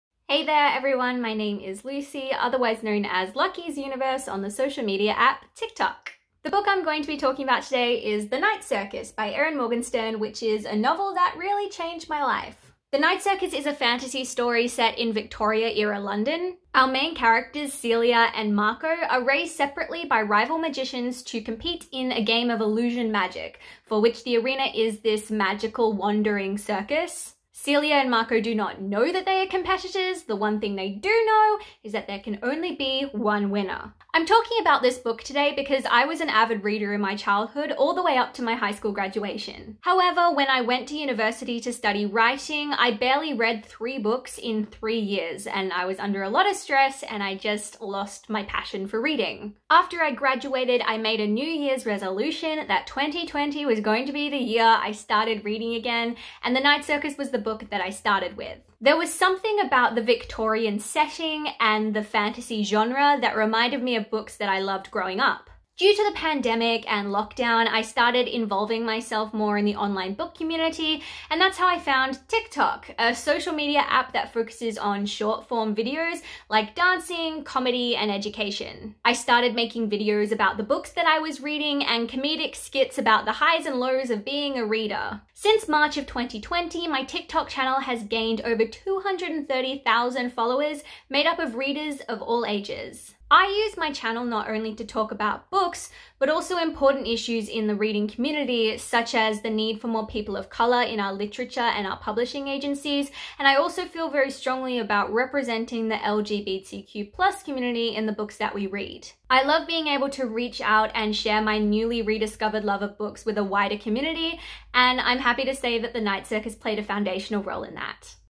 Let's Read Together: Lucy Donaldson
In this series of short videos, key figures in the world of literacy and reading share their reflections on books that have meant a lot to them.

In this fifth video, hear from Lucy Donaldson, Public Library Officer and TikTok enthusiast, who talks about The Night Circus by Erin Morgenstern